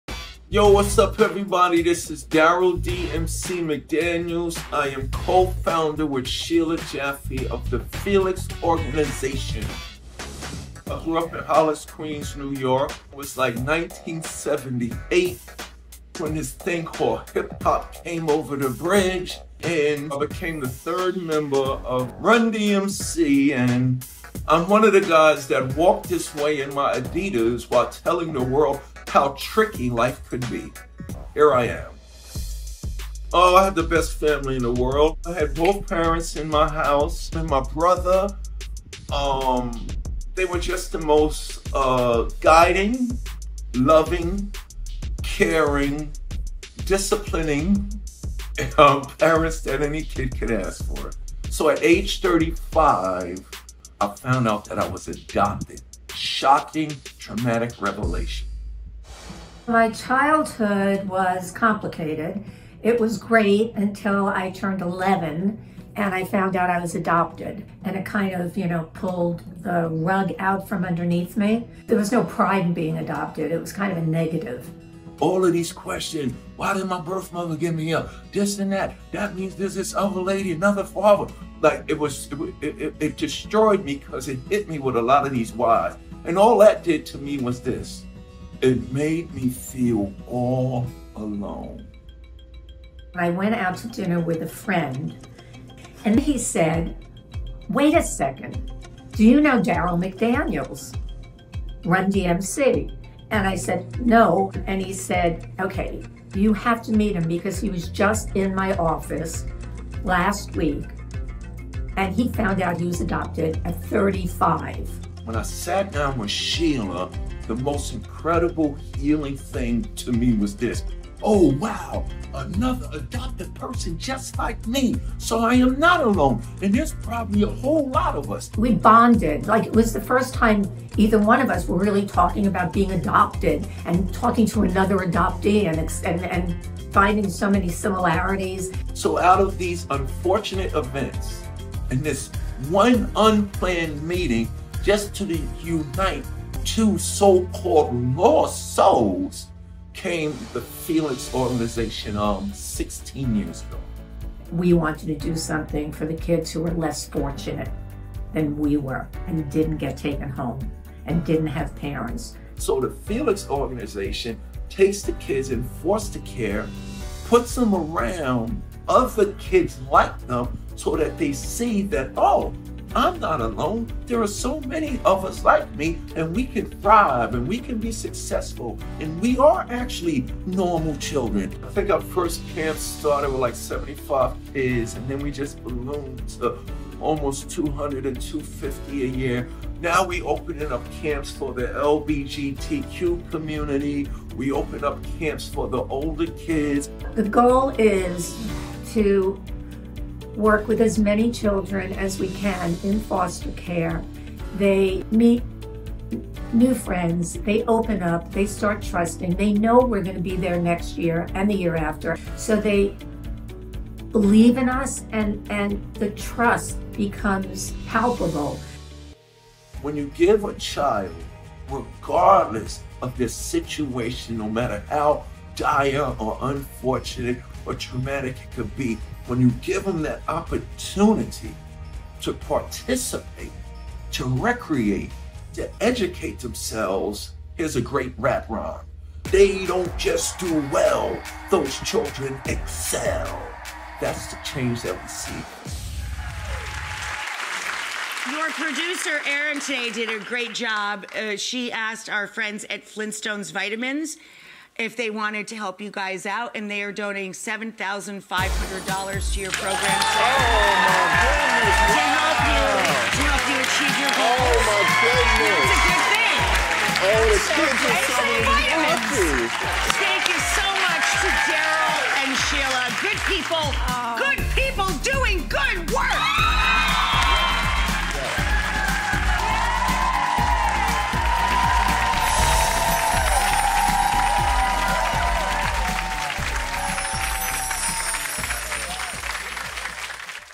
Run-DMC's Darryl McDaniels Helps Foster Children After Finding Out He Was Adopted at Age 35
After finding out he was adopted at age 35, Run-DMC rapper Darryl McDaniels teamed up with fellow adoptee Sheila Jaffe to help foster children in need.